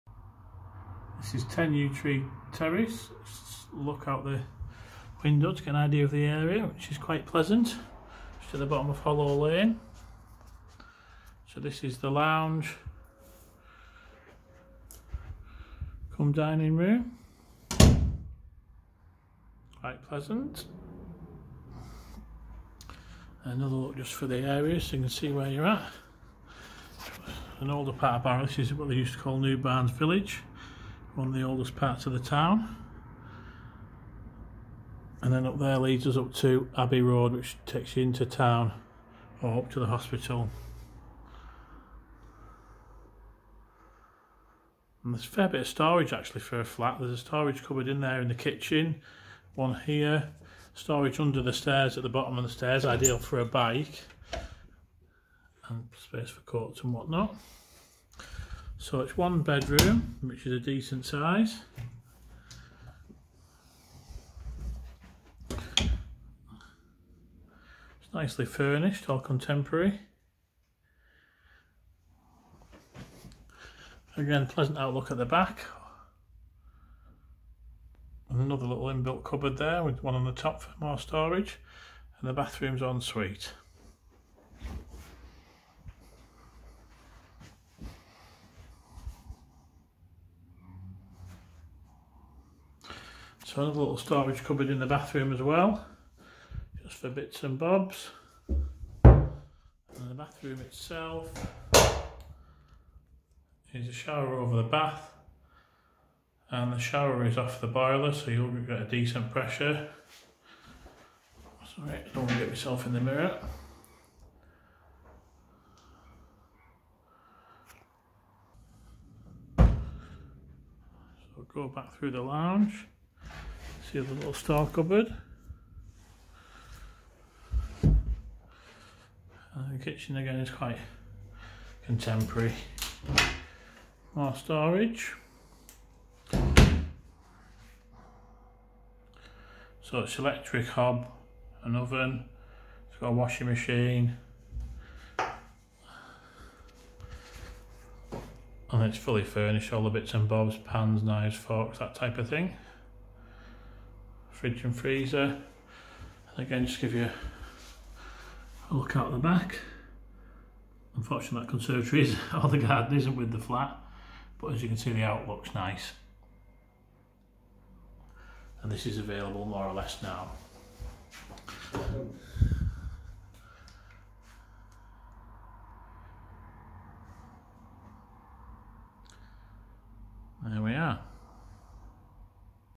10 Yew tree terrace. Pleasant furnished apartment in a nice setting just off Hollow lane £495pcm
* Well presented fully furnished first floor apartment in popular Newbarns village area
* Light and welcoming lounge with pleasant outlook
* One good sized double bedroom with en-suite bathroom
* Modern fitted kitchen
* Small outside storage area
* A neutral decor throughout and in good order
* Ideal let for single occupant coming to work in the area

* EPC Rating: Current C, Potential C

£495pcm deposit of £565